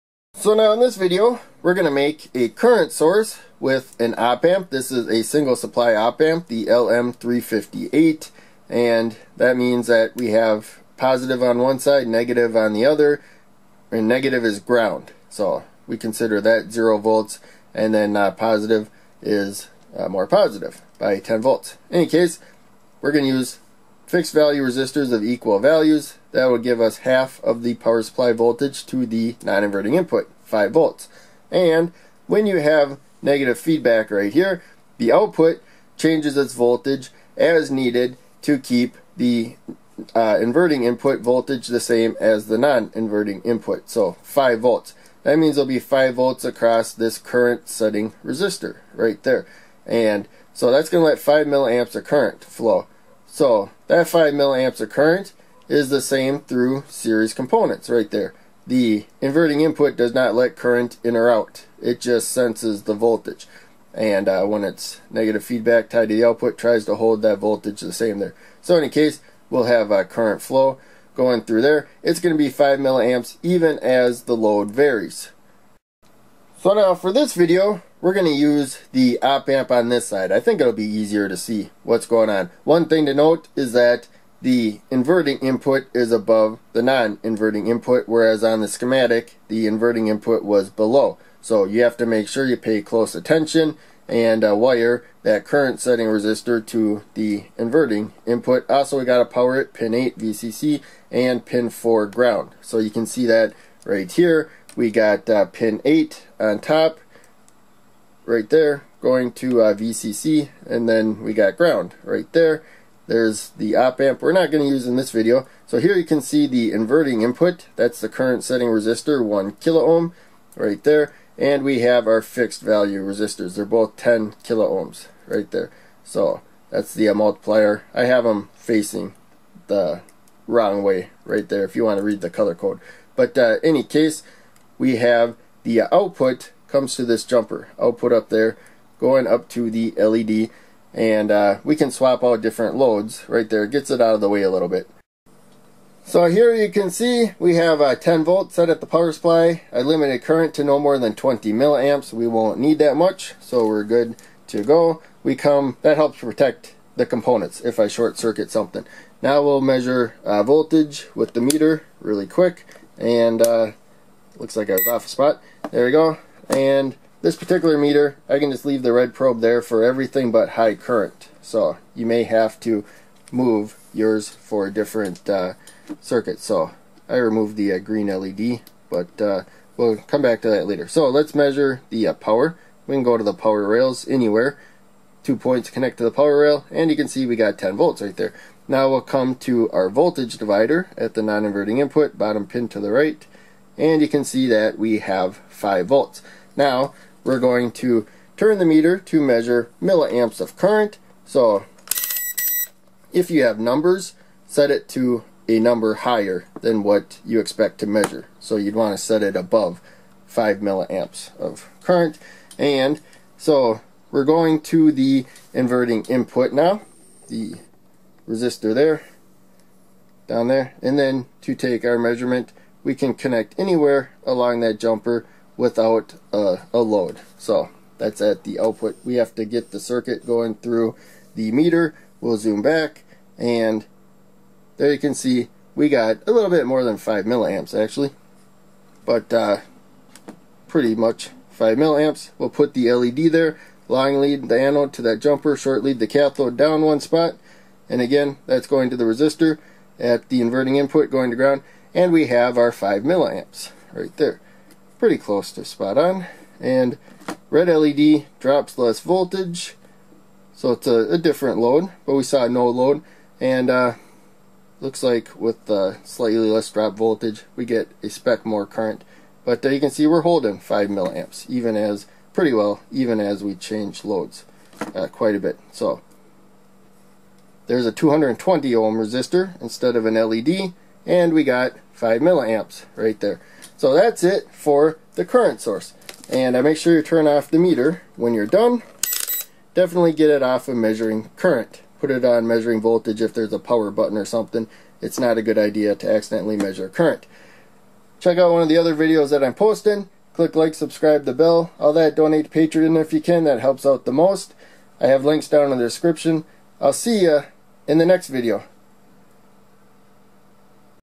5mA current source using single supply op amp LM358 circuit
When using negative feedback (output electrically connected to inverting input), the op amp tries to keep the inverting input (-) at the same voltage as is at the non inverting (+) input. You can use this voltage to put across a resistor going into ground. That makes the output, and any other components added to it that are headed to - input, pass the same current as the current setting resistor with a fixed voltage across it.